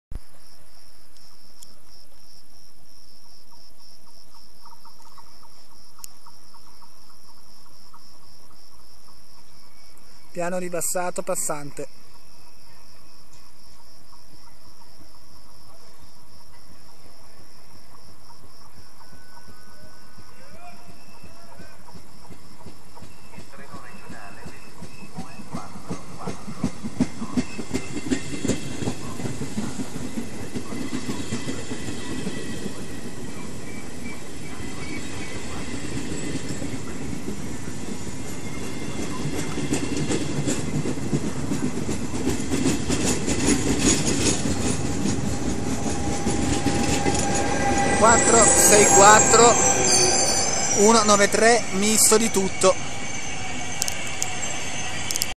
P.R. passante con 464.193 mix.mp4
ebbene si avete sentito bene: sta loco ha: l'XMPR trenitalia; vivalto; TI.LO fate 'n pò voi.